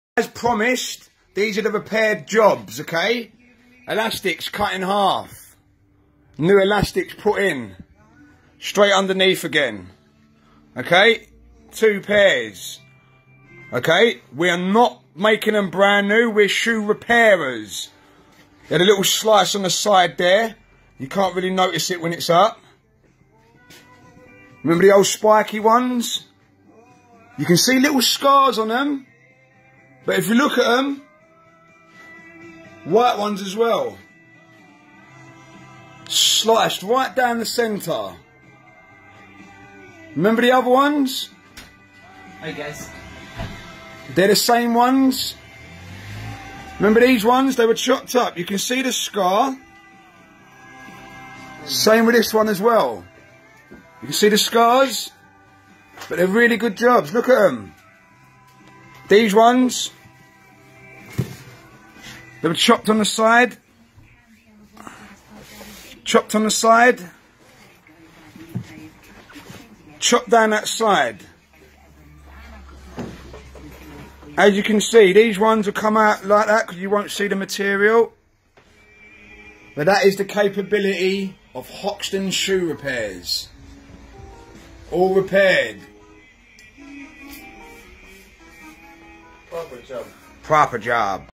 This is the repaired results after Hoxton shoe repairs repaired the designer sho
After £10,000 of Damage by ANGRY Girlfriend. Don't Cheat Guys.. See repairs done at Hoxton Shoe Repairs. London.